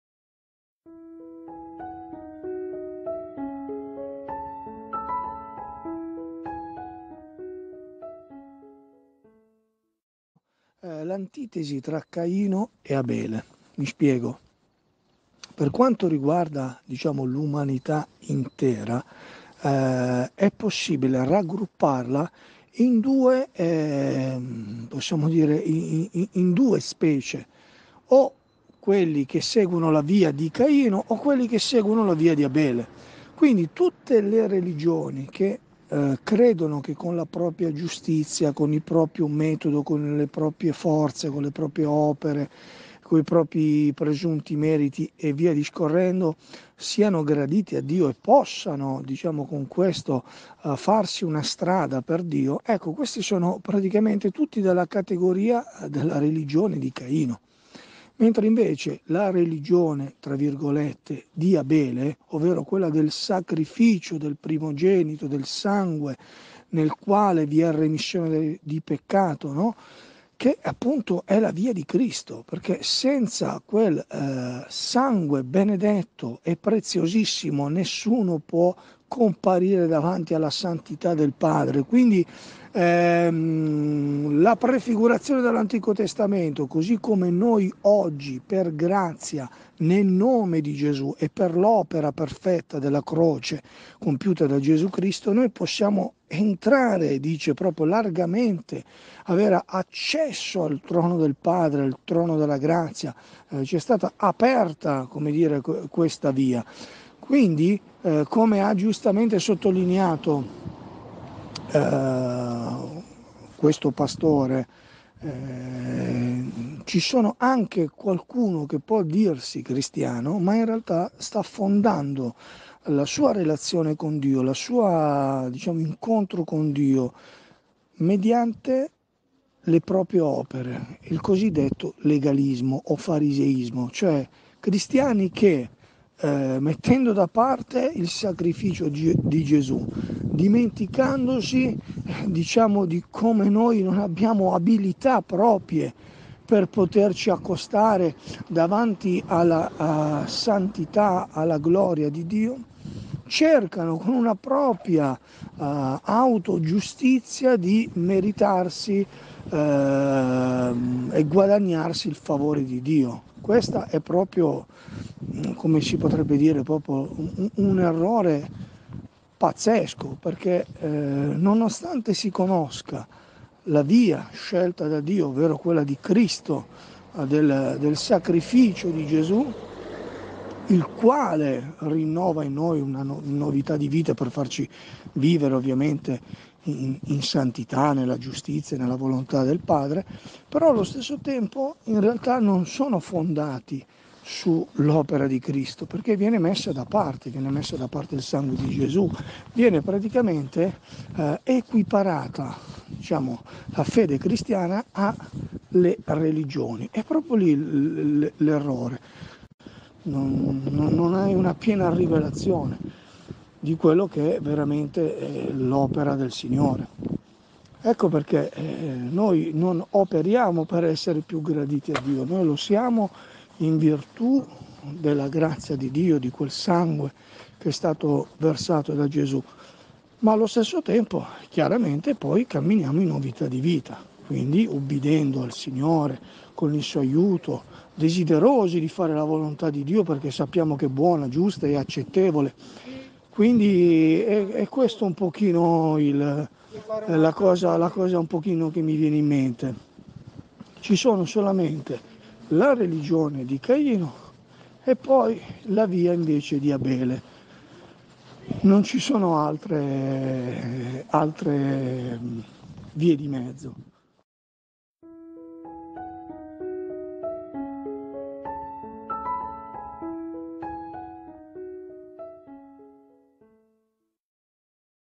LA VIA DI ABELE E LA VIA DI CAINO (Riflessione di Daniele M.)
"E avvenne, di li a qualche tempo, che Caino fece un offerta di frutti della terra all’Eterno; e Abele offerse anch’egli dei primogeniti del suo gregge e del loro grasso. E l’Eterno guardò con favore Abele e la sua offerta, ma non guardò con favore Caino e l’offerta sua. E Caino ne fu molto irritato, e il suo viso ne fu abbattuto"
(Genesi 4:3-5)

CONFORTO CRISTIANO